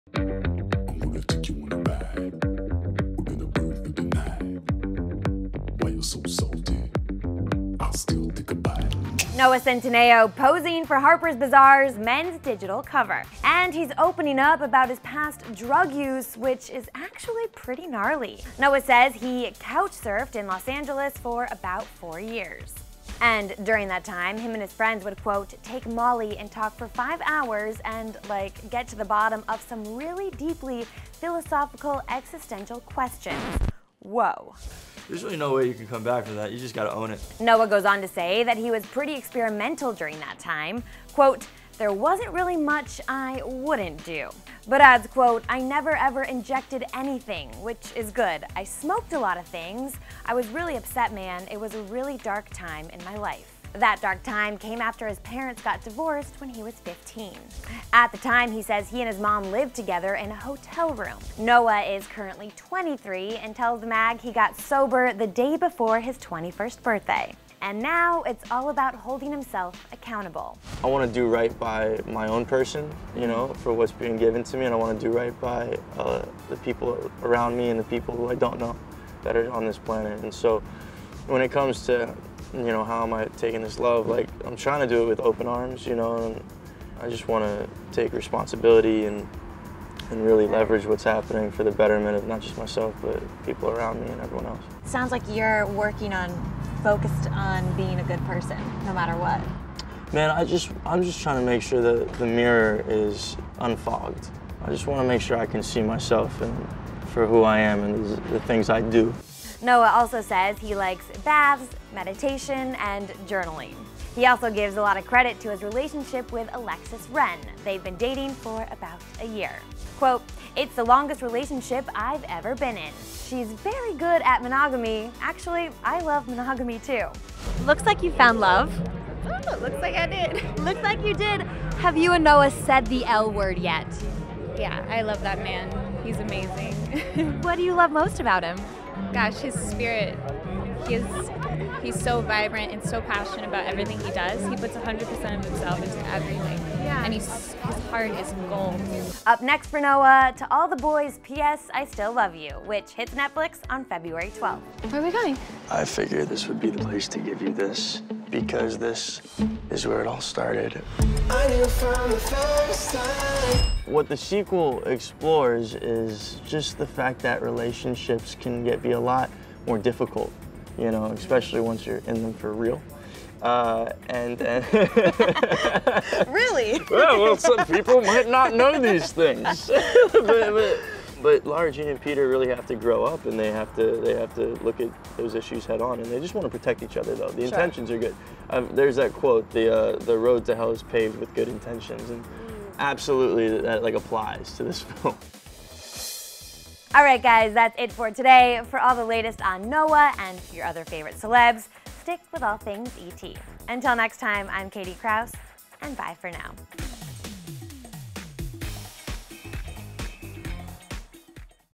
Noah Centineo Talks DRUG Use
Noah Centineo covers HarpersBazaar.com Men's digital cover and opens up about what his life was like before he got sober.